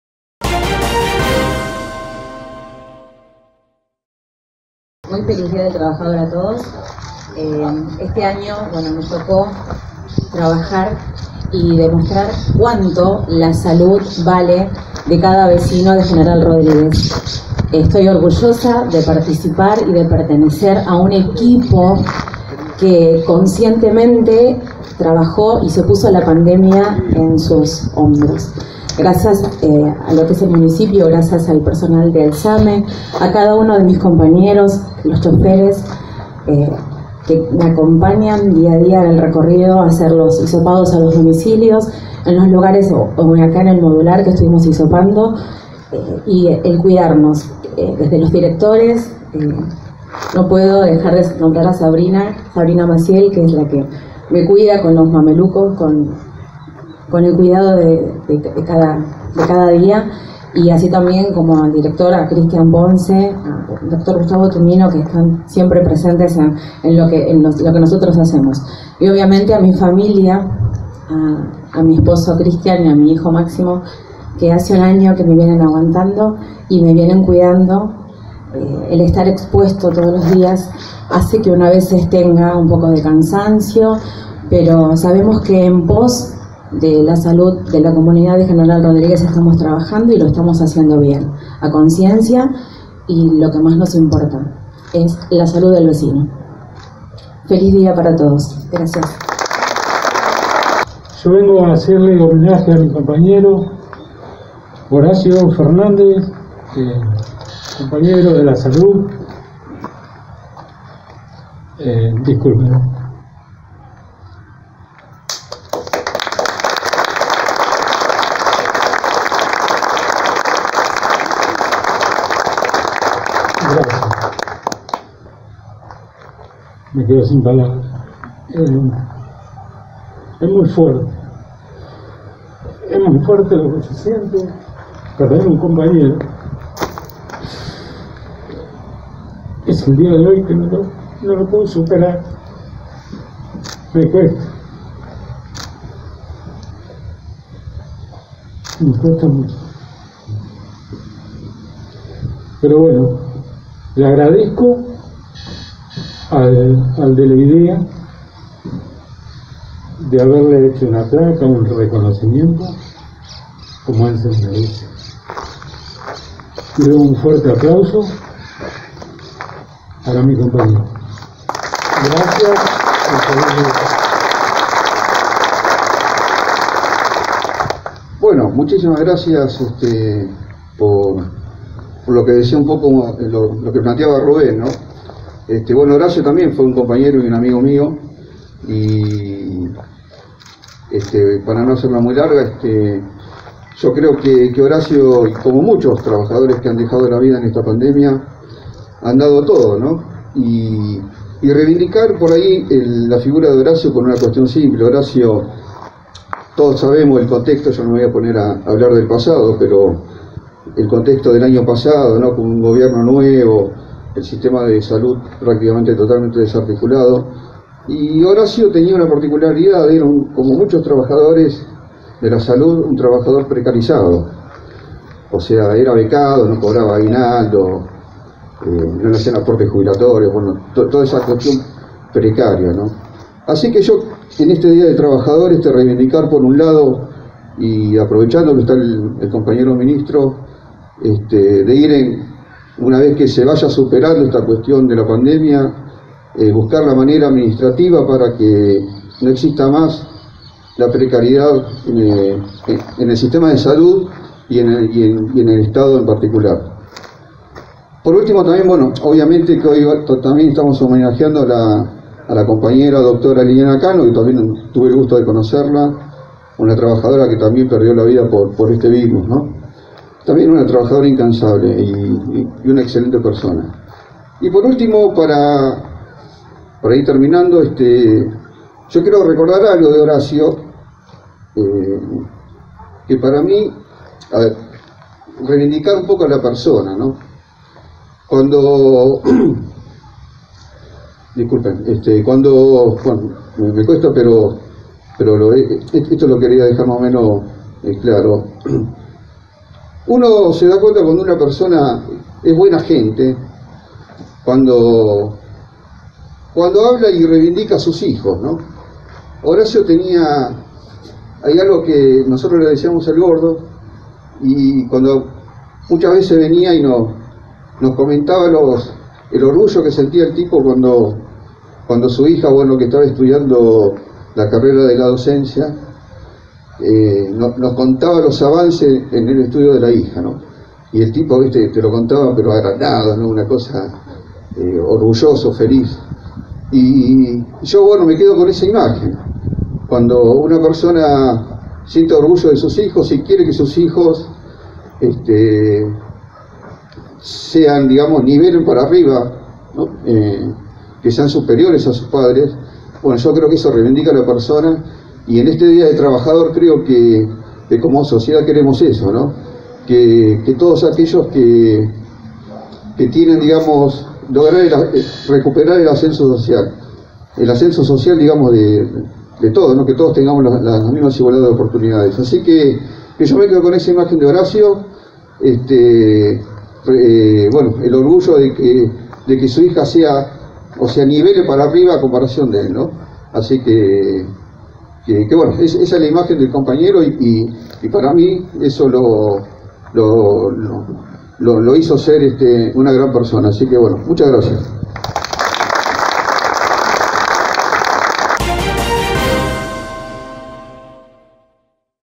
Por la segunda ola, habilitaron el hospital modular de General Rodríguez
La actividad se realizó durante la mañana del pasado sábado 1º de mayo, en la puerta del centro sanitario. Hubo homenaje a trabajadores fallecidos como consecuencia del coronavirus